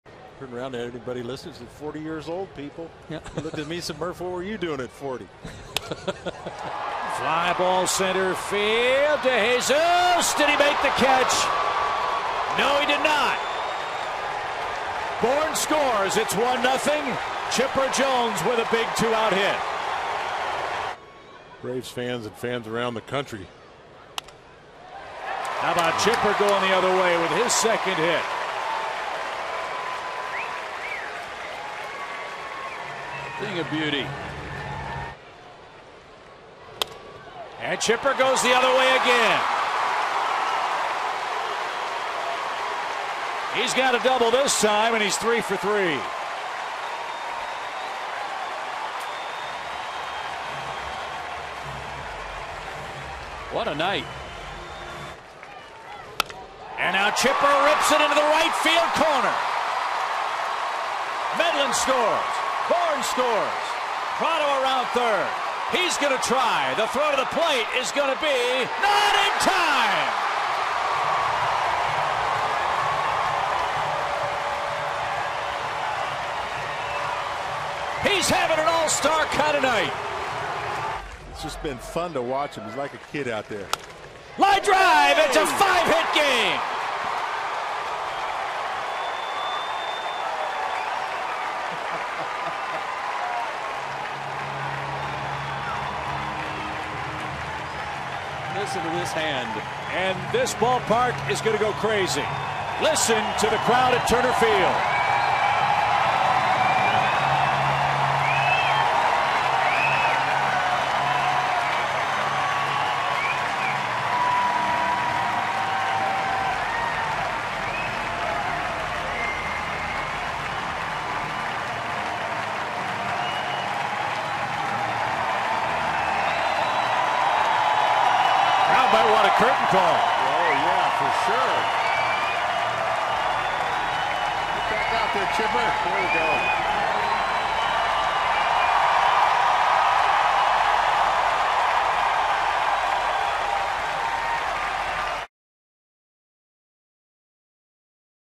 CHC@ATL: Chipper has five hits, exits to curtain call
7/3/12: Chipper Jones enjoys his first five-hit night since 2002, driving in four runs before exiting in the eighth to a standing ovation